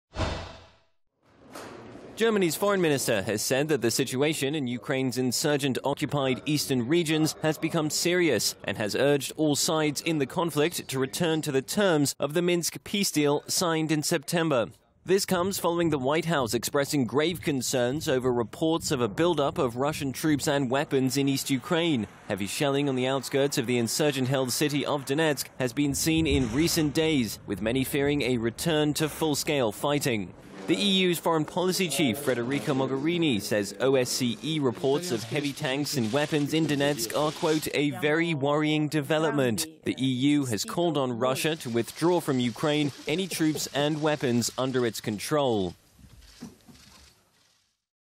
East Ukraine Escalation: German FM says situation 'serious' and urges return to Minsk ceasefire
Germany's foreign minister has said that the situation in Ukraine's insurgent-occupied eastern regions has become 'serious' and has urged all sides in the conflict to return to the terms of the Minsk peace deal signed in September.
The EU's foreign policy chief Federica Mogherini says OSCE reports of heavy tanks and weapons in Donetsk are "a very worrying development." The EU has called on Russia to withdraw from Ukraine any troops and weapons under its control.